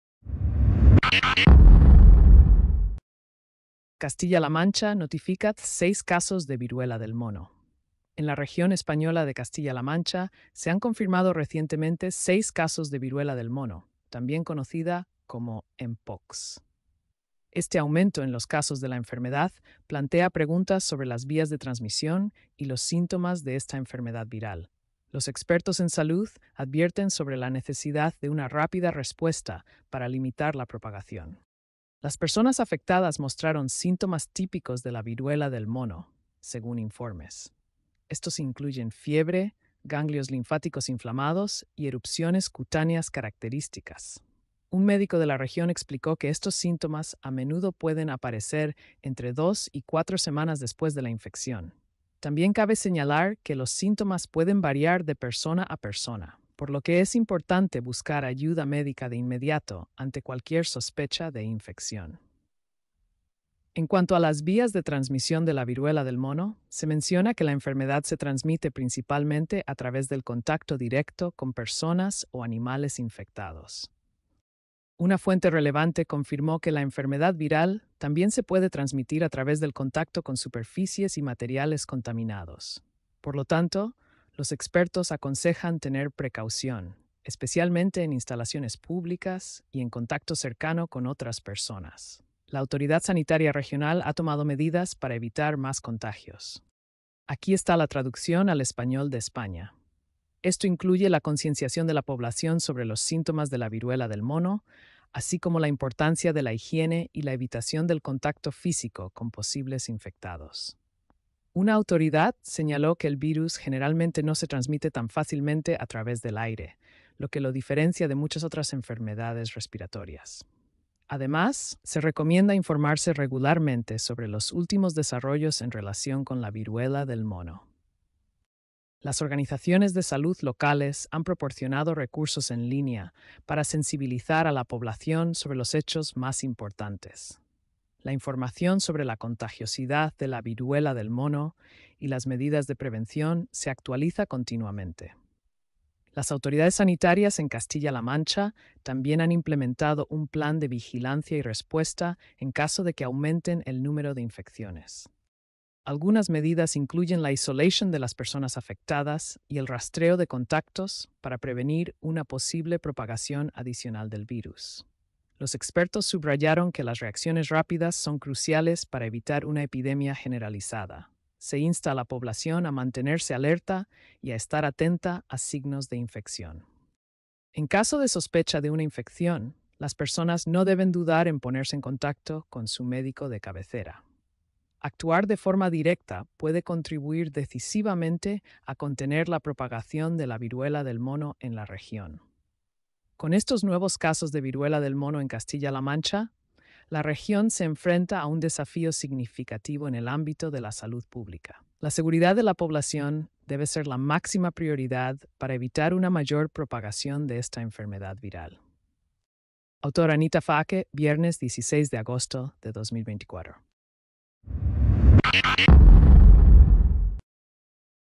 Seis casos de viruela símica en Castilla-La Mancha. (Podcast)
En Castilla-La Mancha se confirmaron seis casos de mpox. AI-generated content